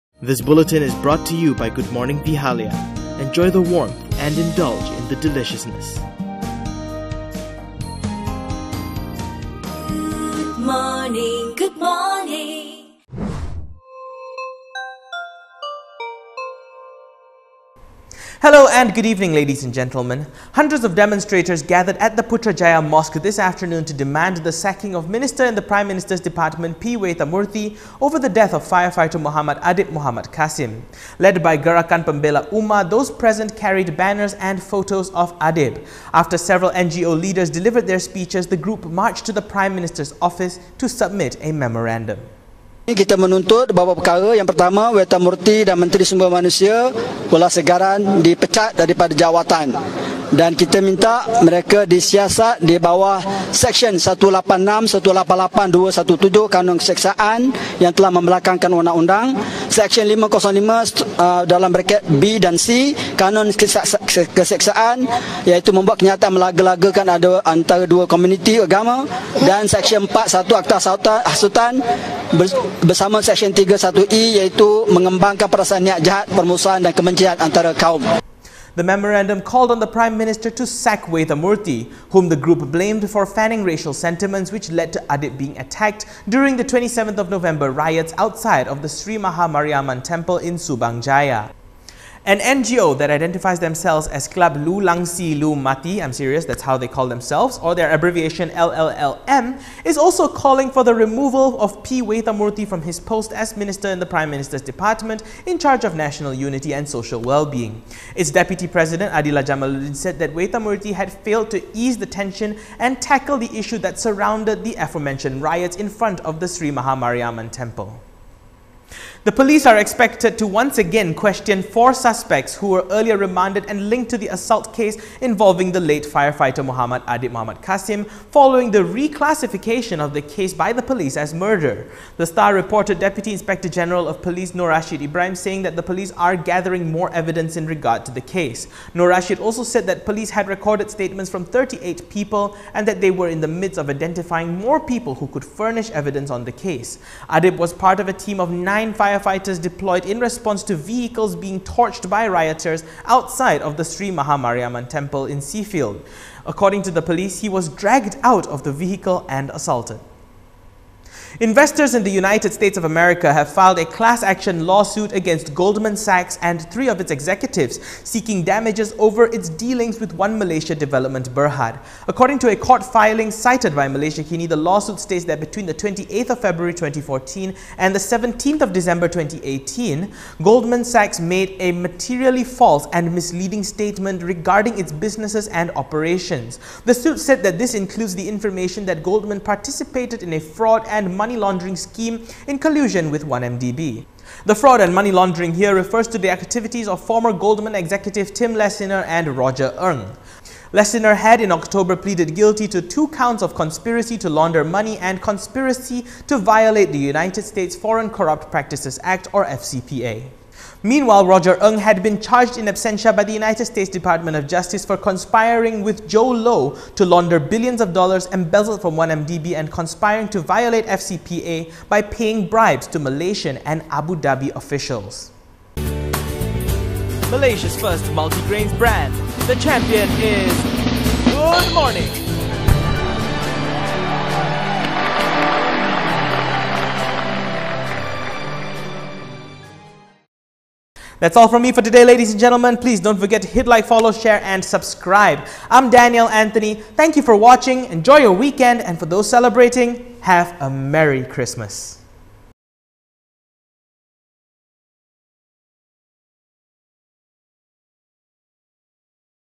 Hundreds gather to demand Waytha's sacking | KiniFlash - 21 Dec
Hundreds gathered at the Putrajaya mosque this afternoon to demand the sacking of Minister in the Prime Minister's Department P Waythamoorthy over the death of firefighter Muhammad Adib Mohd Kassim.

Led by Gerakan Pembela Ummah, those present carried banners and photos of Adib.

After several NGO leaders delivered their speeches, the group marched to the Prime Minister's Office to submit a memorandum.